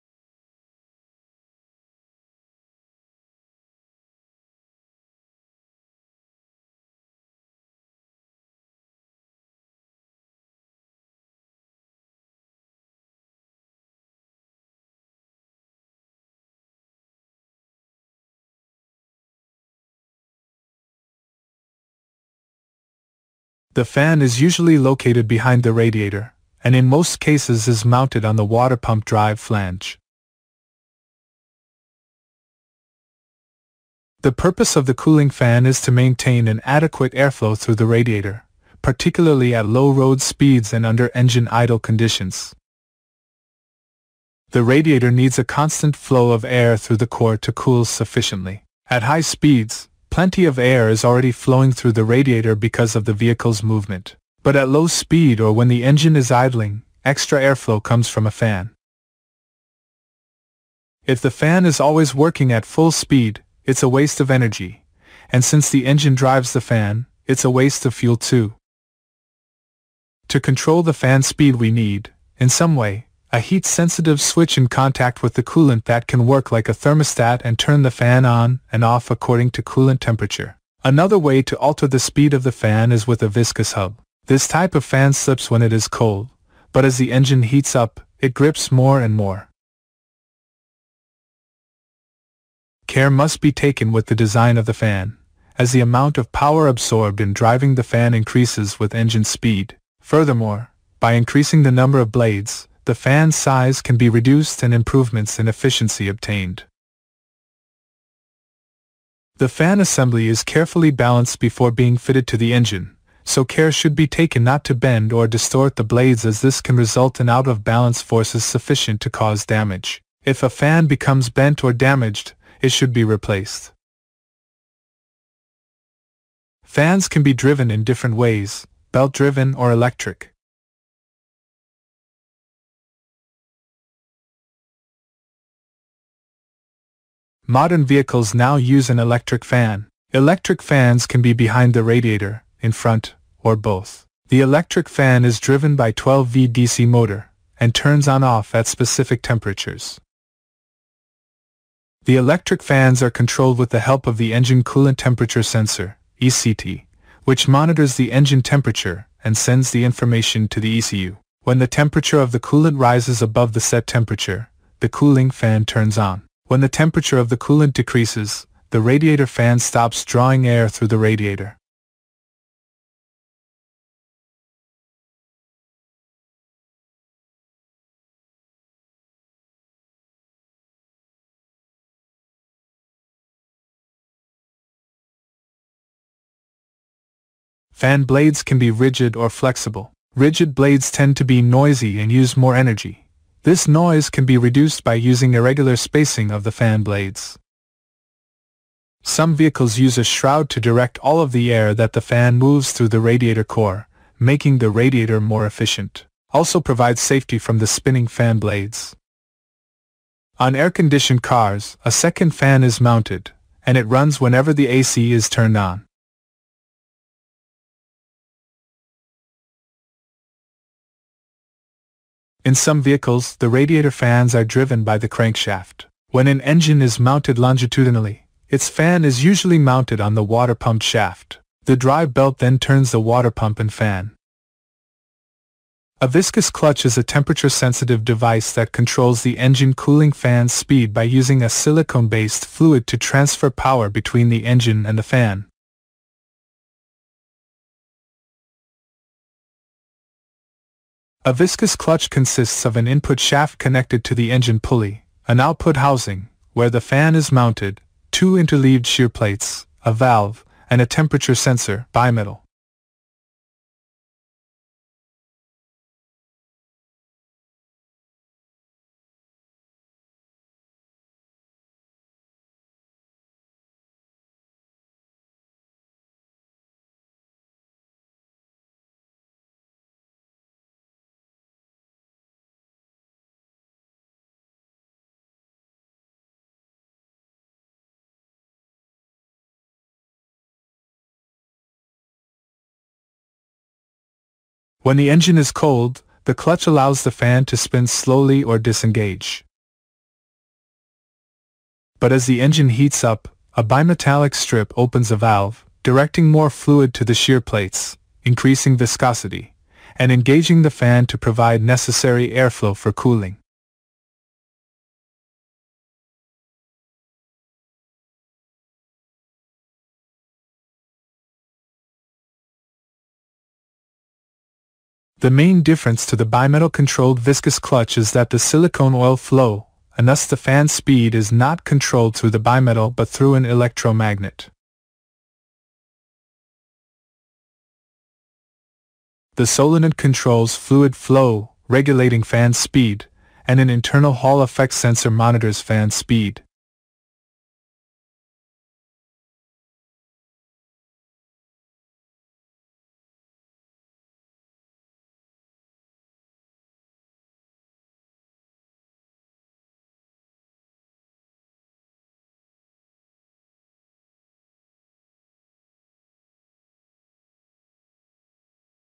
Engine Cooling Fan
• The cooling Fan (Introduction) [fan location / purpose of the fan / fan
        control / fan design /fan assembly balance / fan drives]
• The Electric Fan [location / fan speed control / Parts / AC fan]
• The Viscous Clutch Fan [ parts / fan control]
• Electro-Viscous Cutch Fan [ parts / fan control]